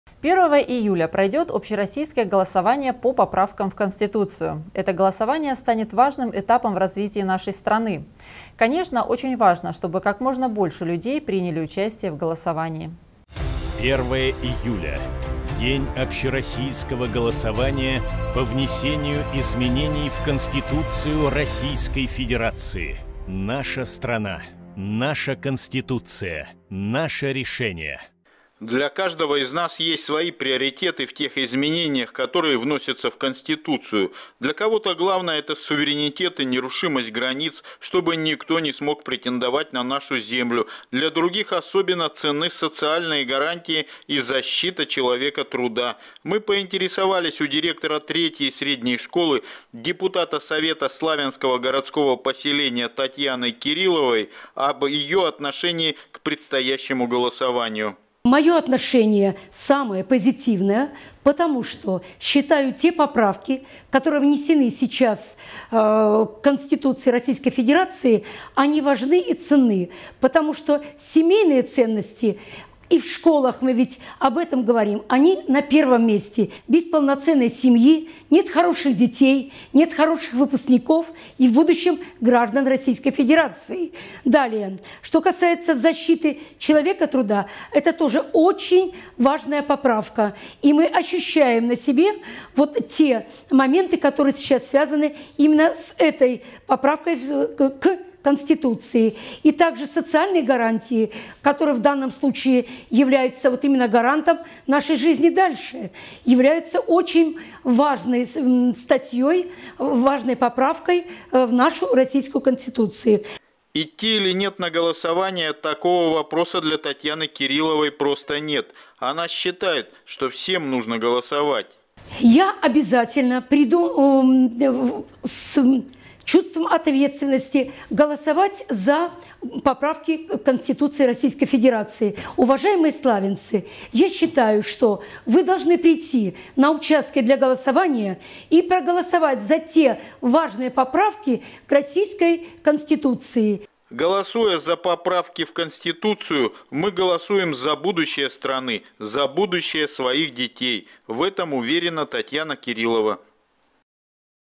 Важный день в нашей истории
1 июля пройдет общероссийское голосование по поправкам в Конституцию. Это голосование станет важным этапом в развитии нашей страны. Конечно, очень важно, чтобы как можно больше людей приняли участие в голосовании